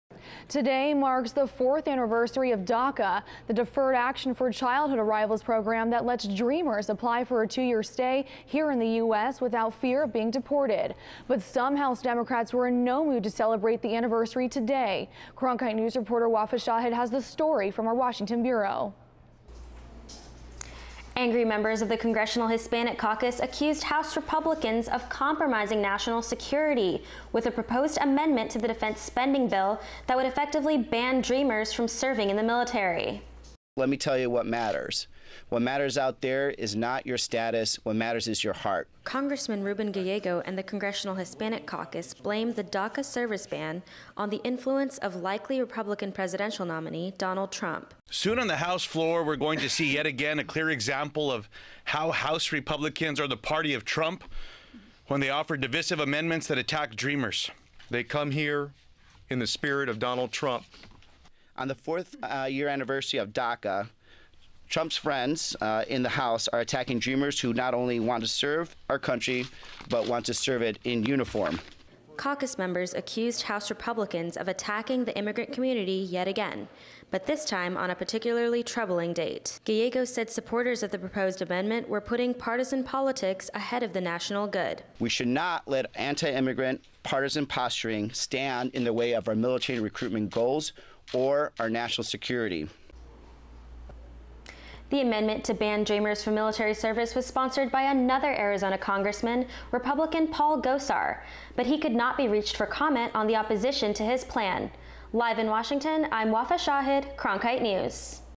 Congressional Hispanic Caucus blames DACA-service ban on Trump's influence | Cronkite News
On the fourth anniversary of DACA, members of the Congressional Hispanic Caucus said Donald Trump and House Republicans are attacking the immigrant community. For more from our Washington D.C. bureau, visit cronkitenews.azpbs.org. (Video by Wafa Shahid/Cronkite News)